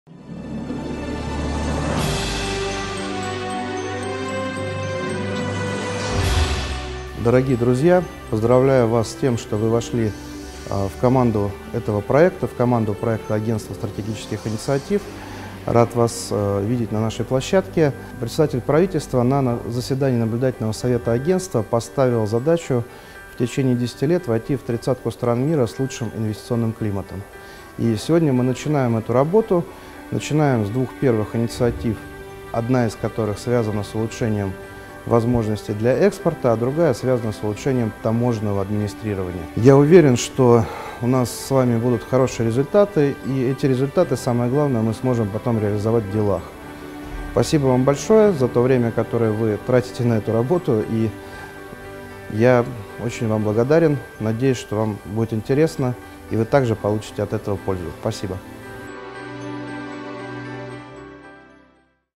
Приветствие участникам краудсорсинга (Андрей Никитин)
Гендиректор АСИ Андрей Никитин поздравил участников проекта и поблагодарил их за сотрудничество.